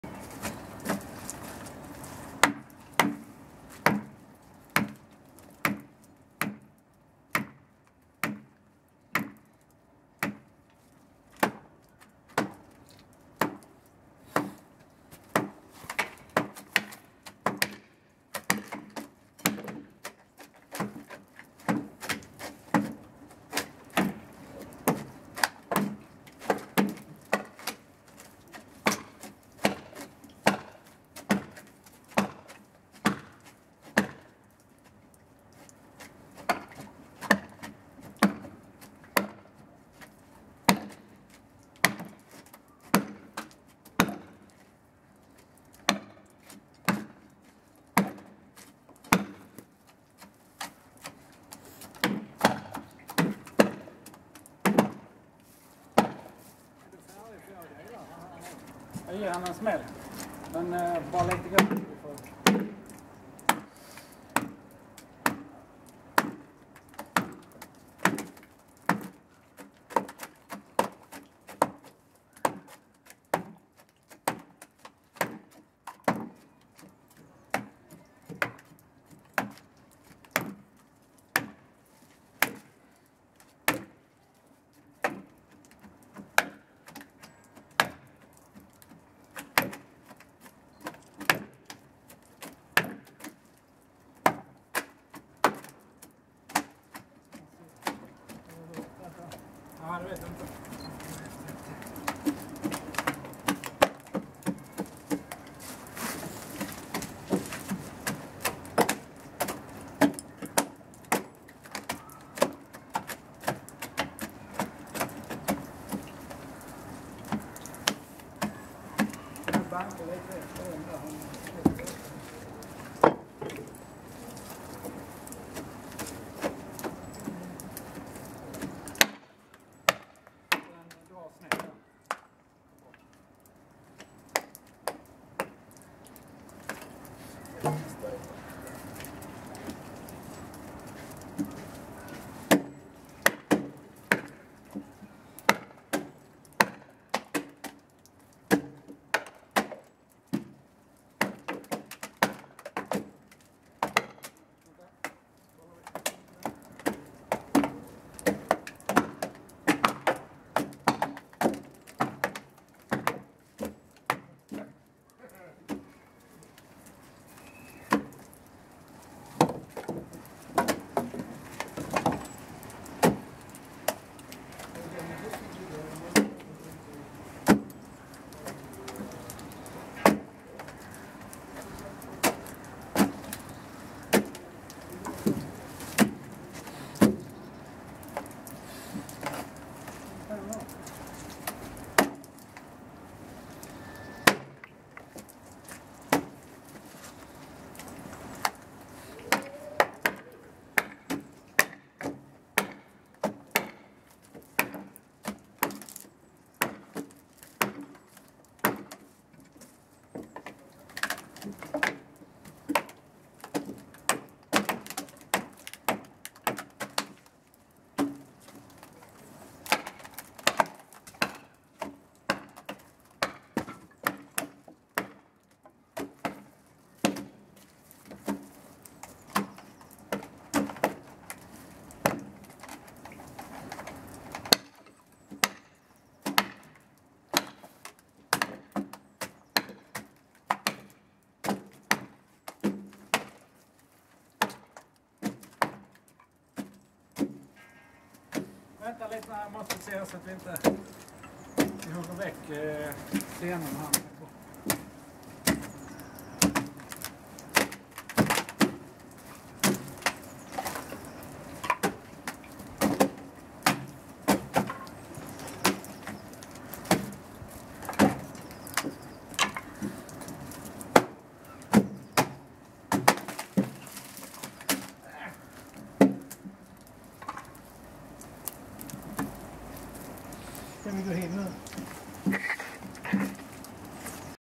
Klyvning av stock till golvplank med yxa och träkilar på Kulturen i Lund
Det sista steget i återuppbyggnaden av Uranäsboden på Kulturen är golvet. Vi återanvänder så mycket som möjligt av de ursprungliga golvplanken, men vi måste också ta fram nya. Det gör vi på det samma sätt som man gjorde när Uranäsboden byggdes i början på 1700-talet - vi klyver stockar med hjälp av yxa och träkilar. Läs mer på Kulturens hemsida